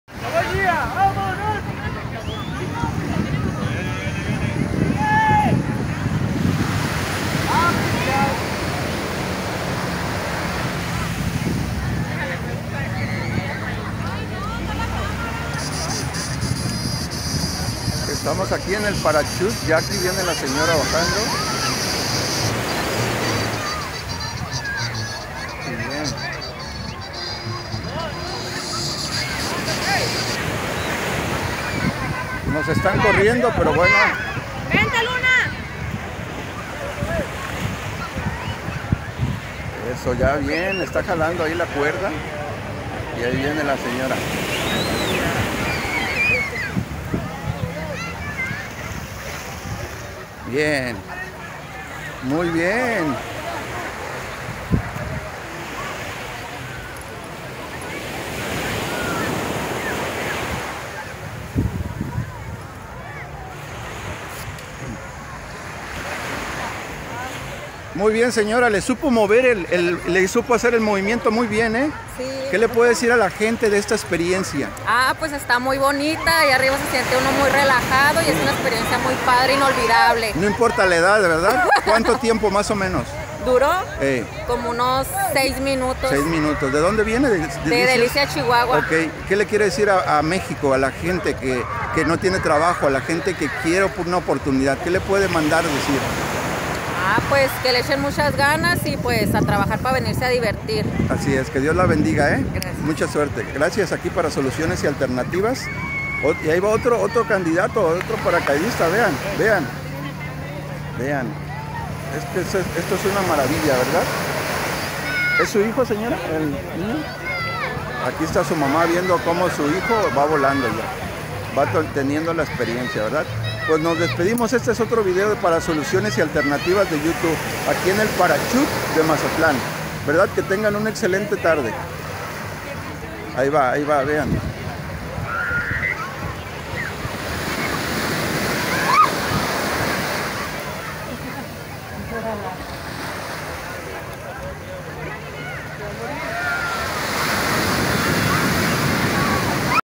Experiencias en la playa de Mazatlán, Sinaloa.
El parashute, una gran diversión.